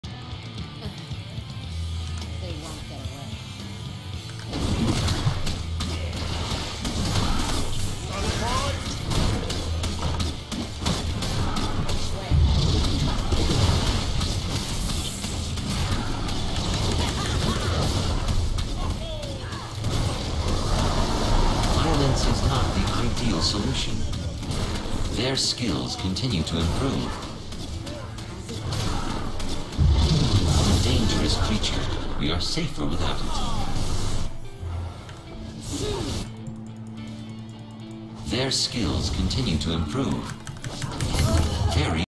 Freia: Pretty much Solo Para
Definitely could have solo'd parasite, basically did. easy mode with this build :) good team zoning too!